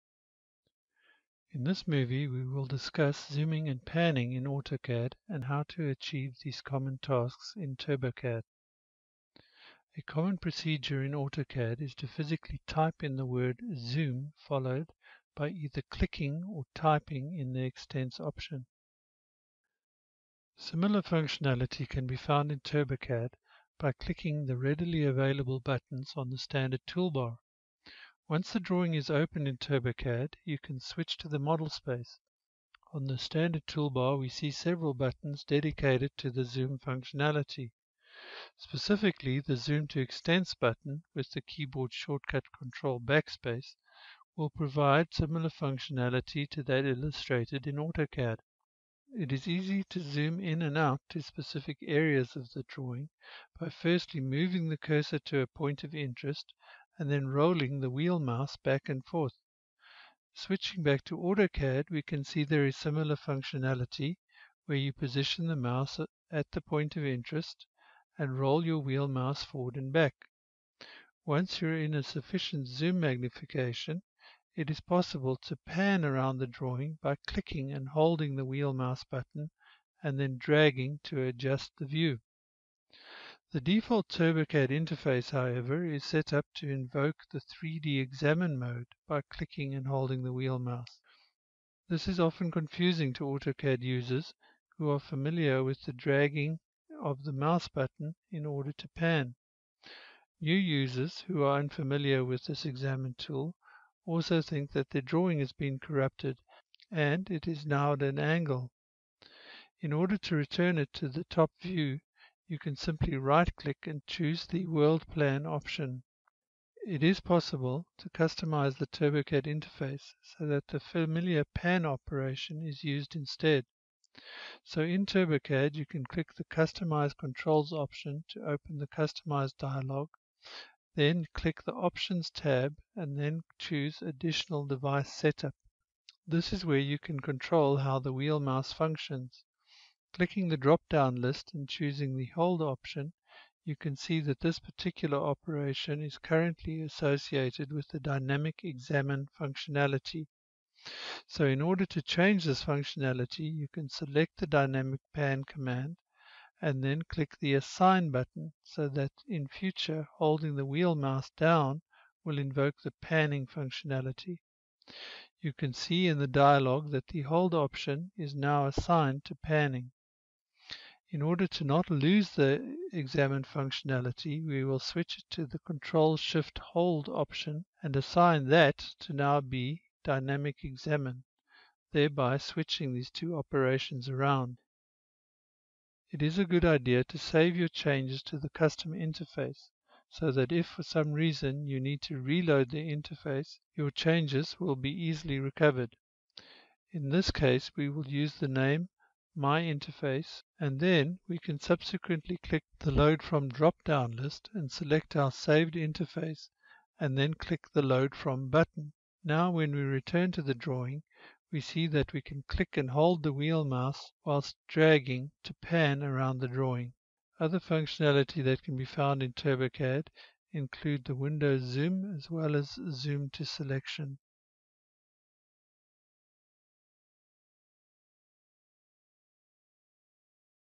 AutoCAD-TurboCAD Zooming and Panning
This movie describes the zoom and panning functionality that is available in TurboCAD and how it compares similar functionality in AutoCAD